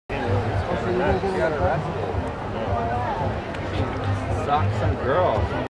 Winter Ave Zoli arrives at the Miss Golden Globe 2013 Par...
Winter Ave Zoli arrives at the Miss Golden Globe 2013 Party in Los Angeles, 11/29/12